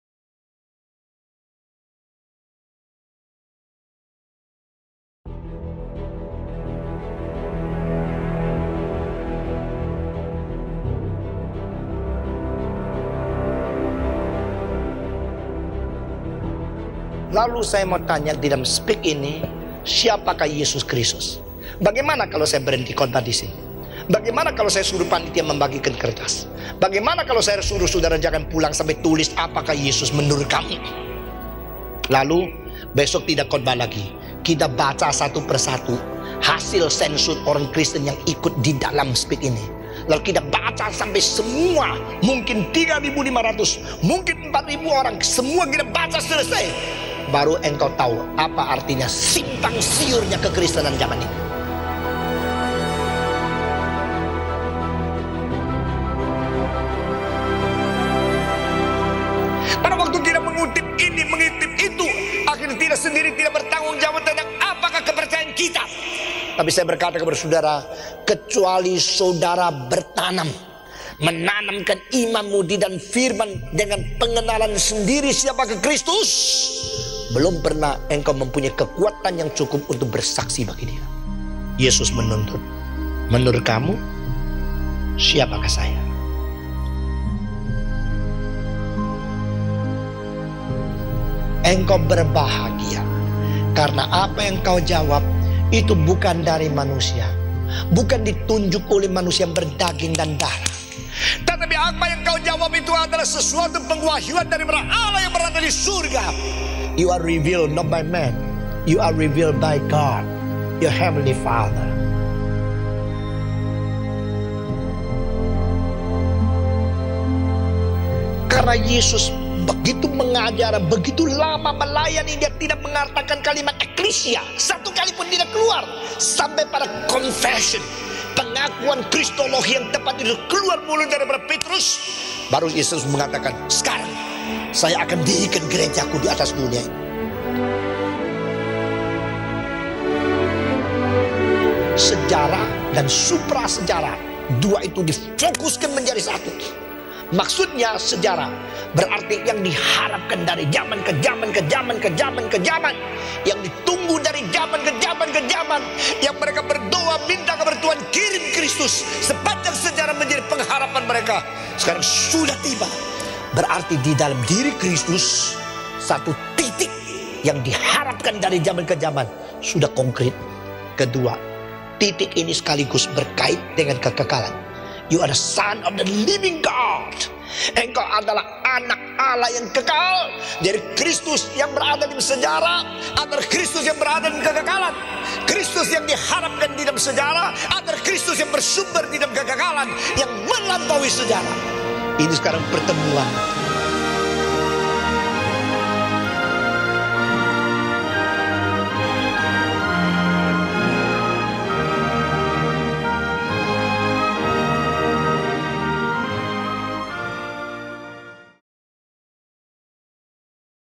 SPIK Kristologi IV 2017 [ENG SUB] [CHI SUB]
Seminar Pembinaan Iman Kristen (SPIK) 2017
KRISTUS dalam Alam Semesta, Sejarah, & Gereja

Pembicara:
Pdt. Dr. Stephen Tong & rekan-rekan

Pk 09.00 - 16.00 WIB
Katedral Mesias, RMCI, Kemayoran

Biaya Pengganti (termasuk makan siang):
Rp 80.000 (Umum)
Rp 40.000 (Mahasiswa)